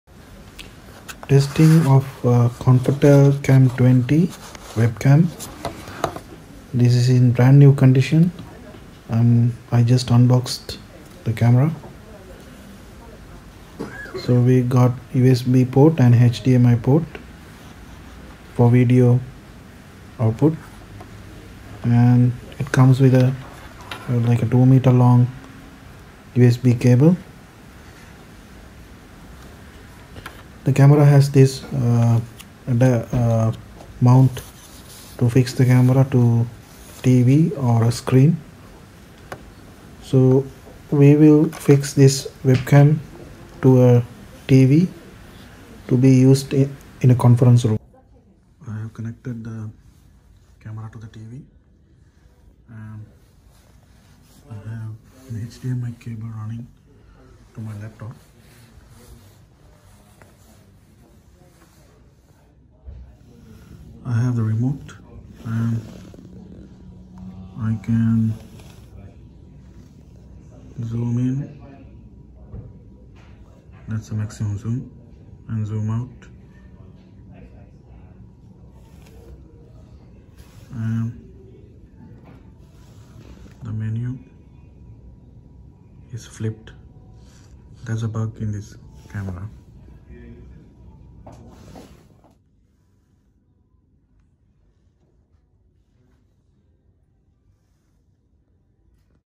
Setting up a video conference room
Konftel cam20 web cam is used. But the cam has a glitch. It shows the menu reversed when used with a video conferencing software but works fine with built-in windows camera app.